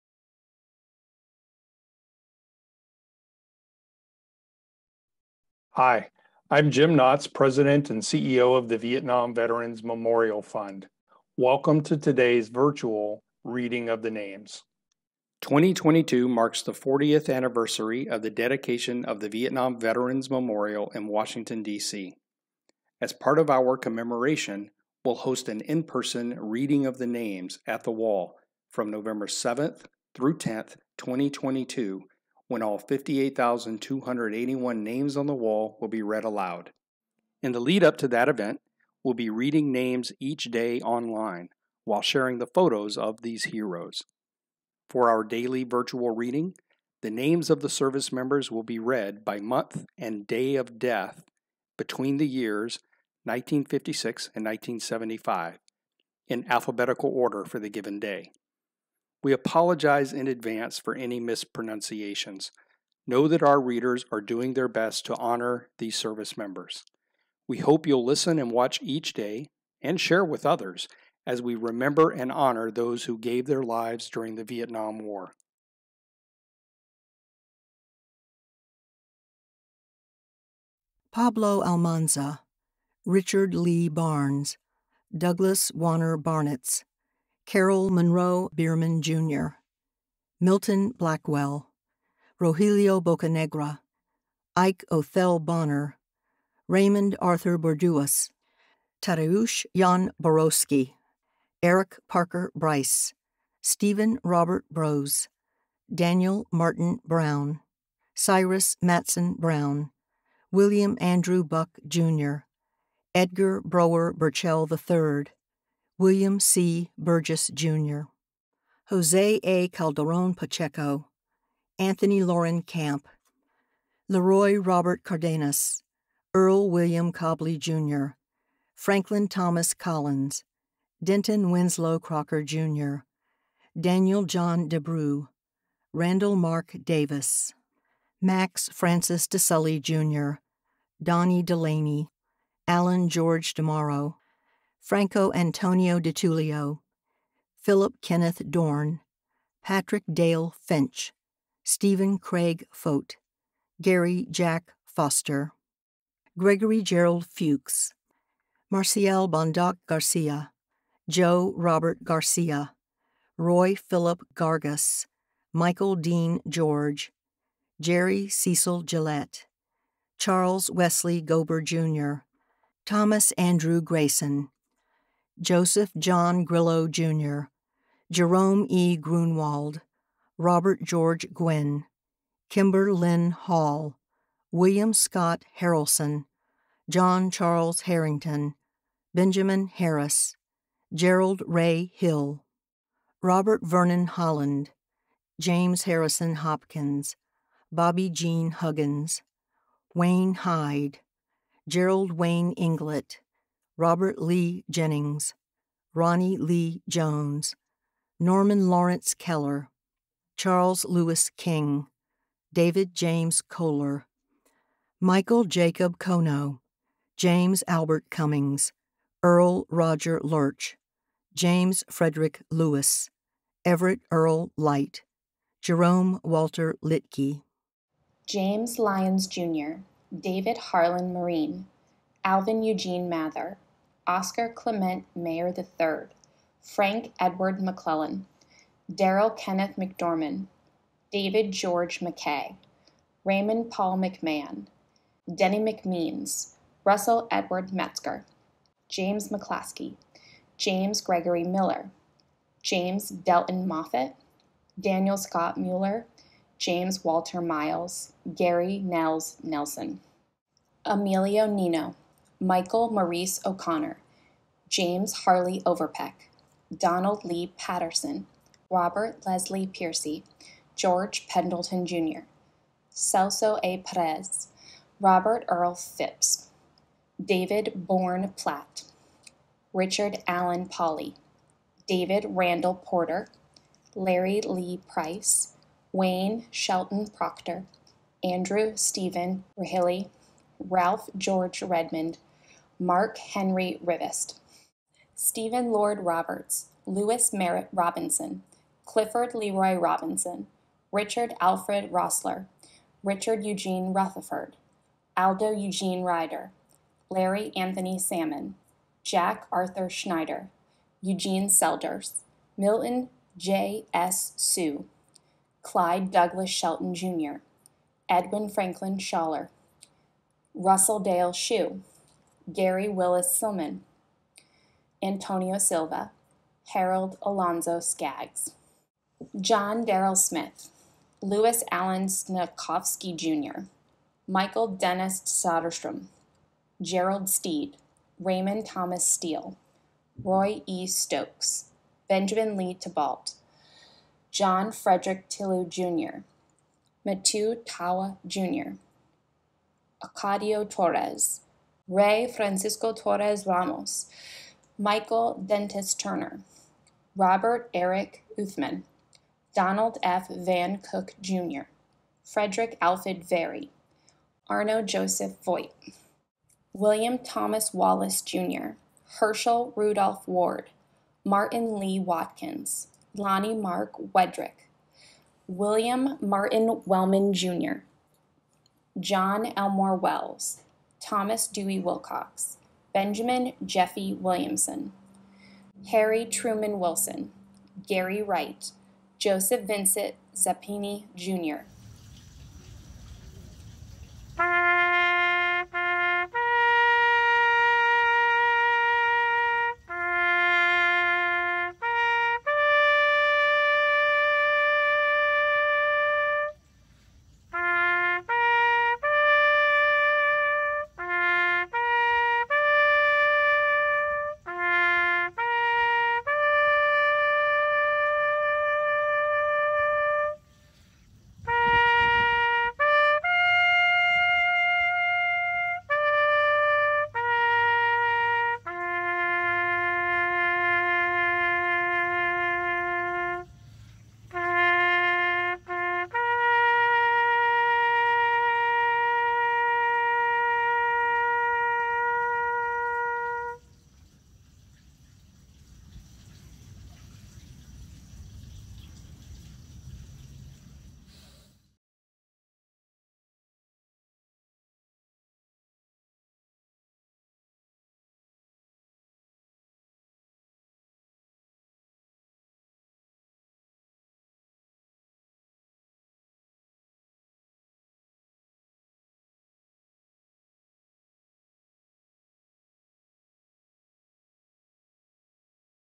Daily Virtual Reading of the Names: June 4
We honor those that died on June 4 between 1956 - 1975 during the Vietnam War. This series is a lead-up to our in-person Reading of the Names event at the Vietnam Veterans Memorial in Washington, DC between November 7-10, 2022.